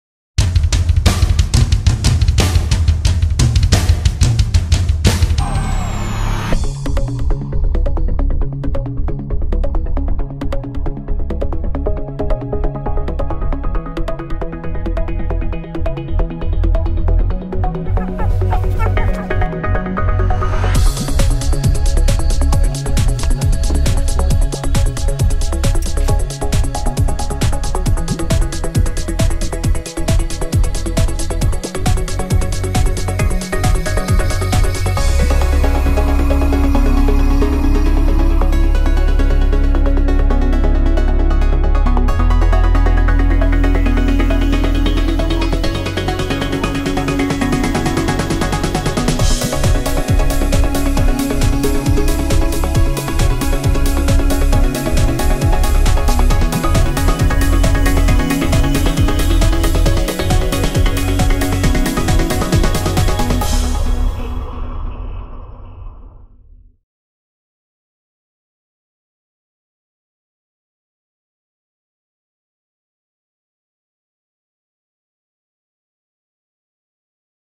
STEPANEK - XTB World Record Challenge 2011: Competition line
STEPANEK - XTB World Record Challenge 2011 Measuring the competition line